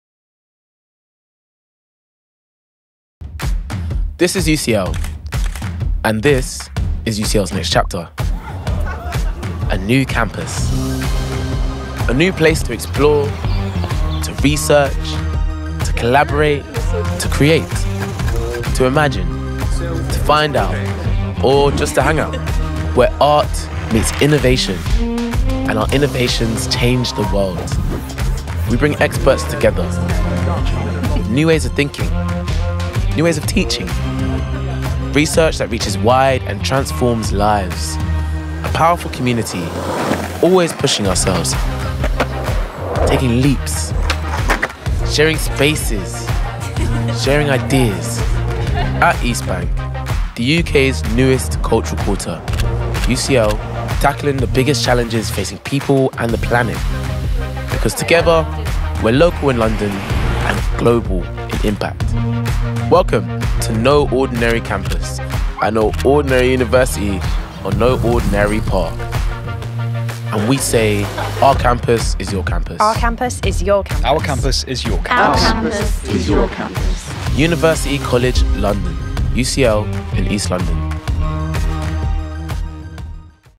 UCL launches its new east London campus
UCL is the first of five world-leading institutions to open at East Bank, the UK’s new culture and innovation quarter.

The interdisciplinary campus is a direct response to the biggest challenges facing people and the planet.

It is our largest expansion in our almost 200-year history, and is designed to accelerate breakthroughs in areas including robotics and AI, ecology, sustainable cities, decarbonised transport, assistive technology, and global health.